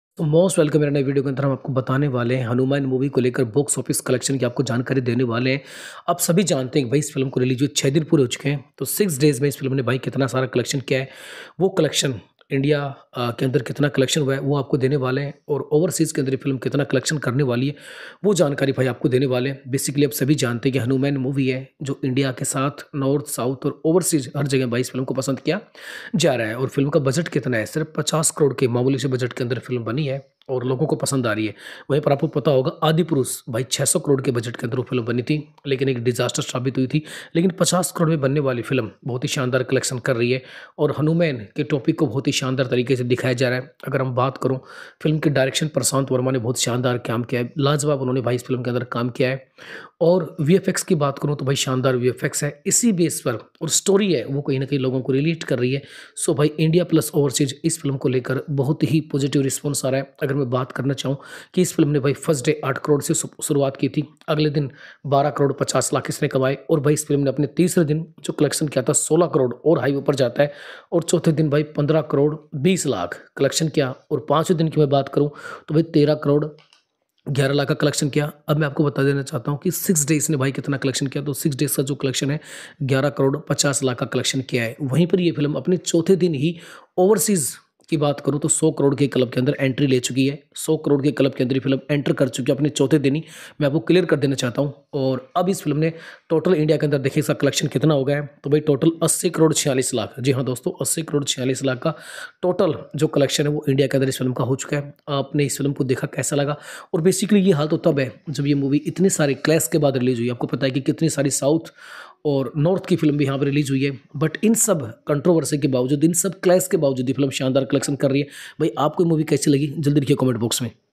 Hanuman Movie Days 6 Box office collections Report||Hanuman Movie Total Box Office Collection
in this video I am going to tell you about Hanuman Movie Box office collections Report about 6 Day collection update.Hanuman is a low budget movie but story and VFX is very good and earing good on Box office.Teja saja played lead role in the Hanuman Movie and review of the Hanuman Movie positive so it is doing good